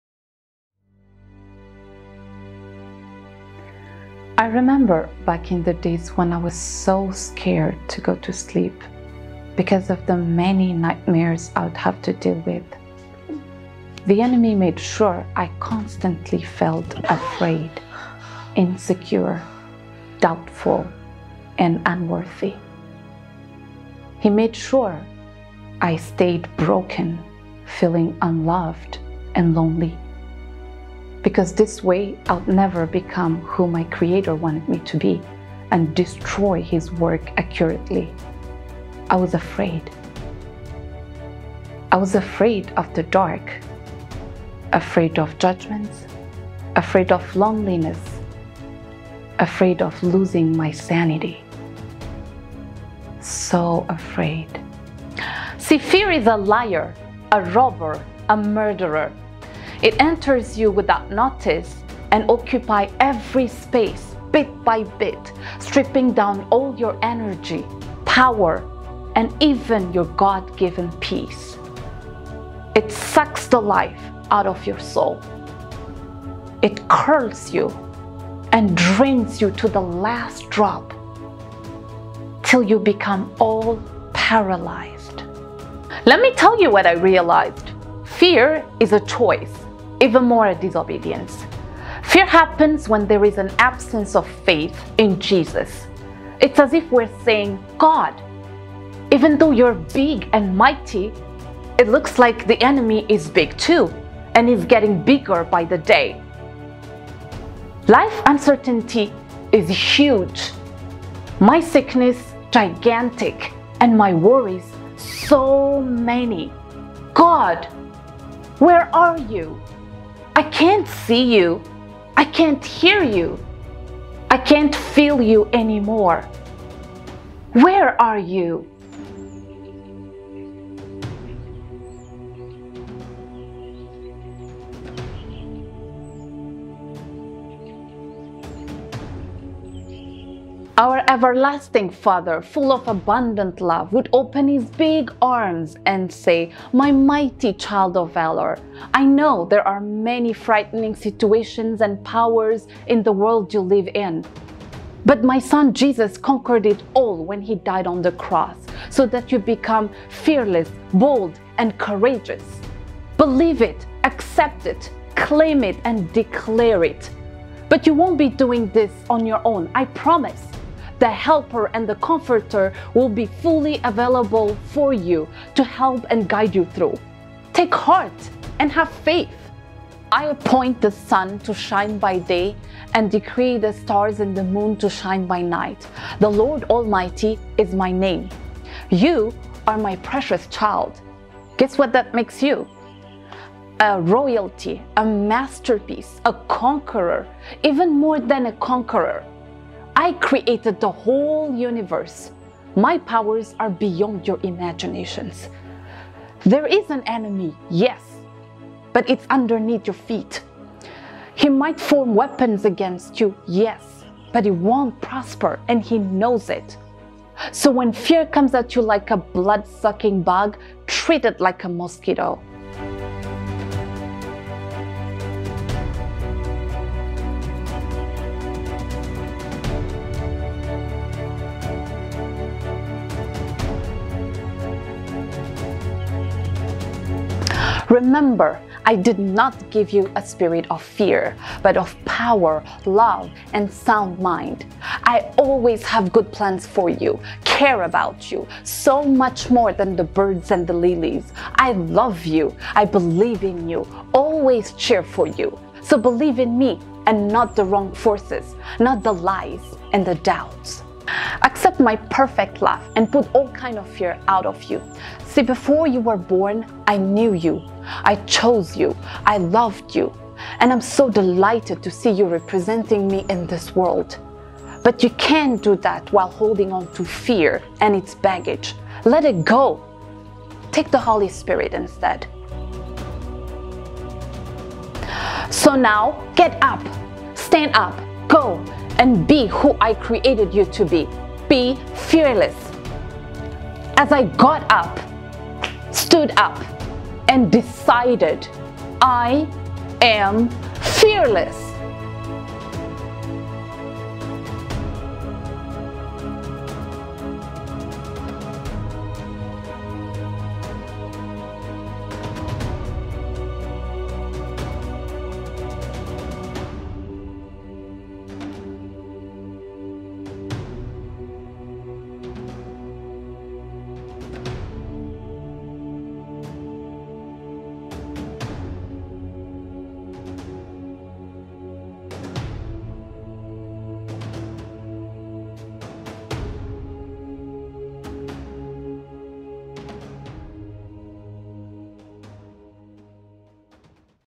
Fear is a Choice
This is a true words of  Ms. Sossena Guetachew about how she overcame fear by standing on the Word of God.